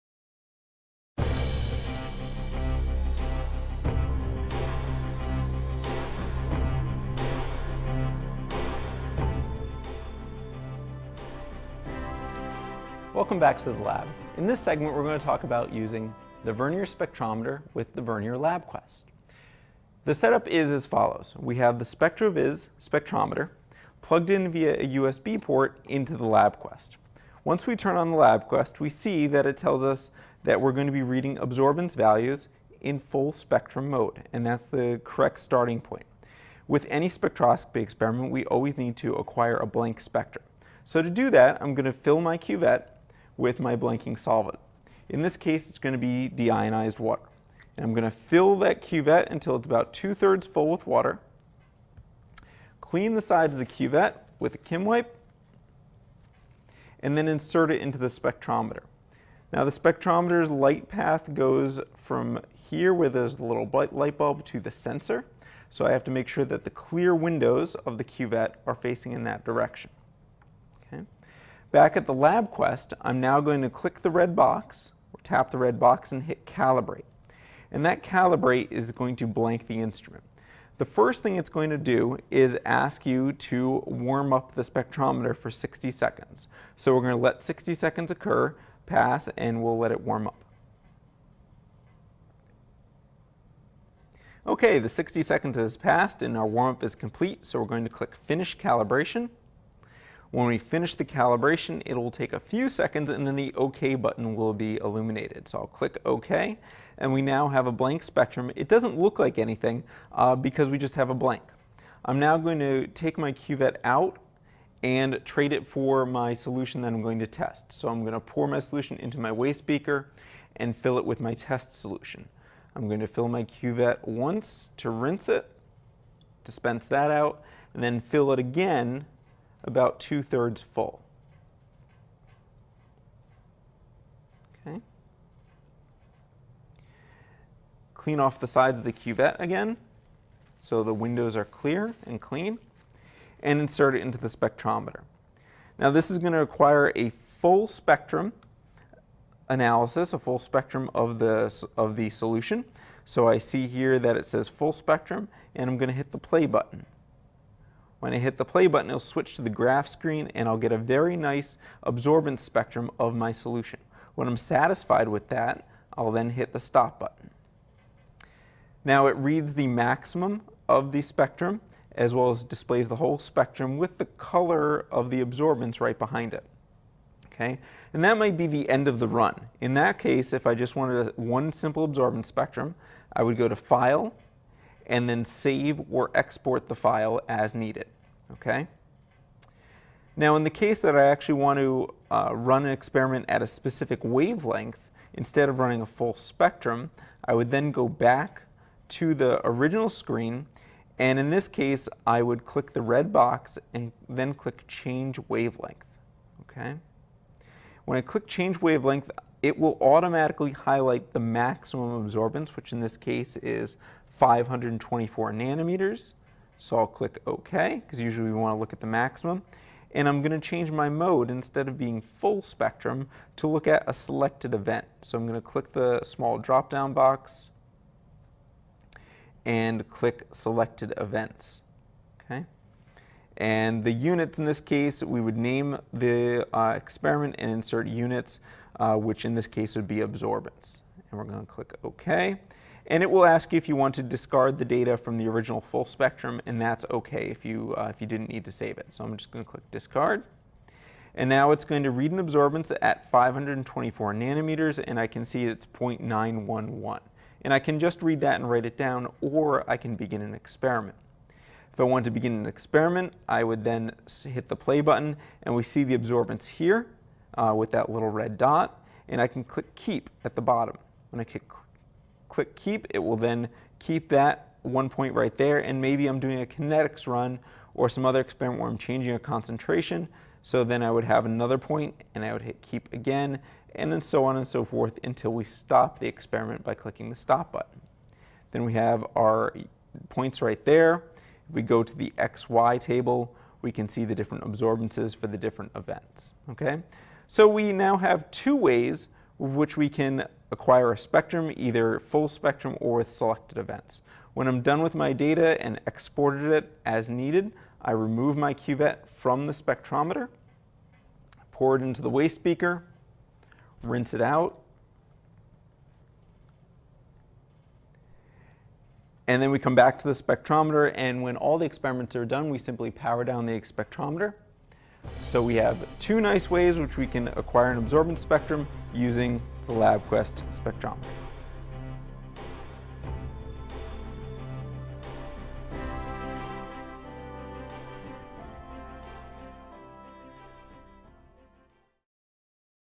Chemistry - How to use the Vernier Spectrometer
SUNY-ESF Assist. Professor Neal Abrams demonstrates how to use a Vernier Spectrometer.&lt;br /&gt;&lt;br /&gt;Content produced by Christopher Baycura for the SUNY College of Environmental Science and Forestry (SUNY-ESF).  Copyright Neal Abrams 2009.  Used with permission.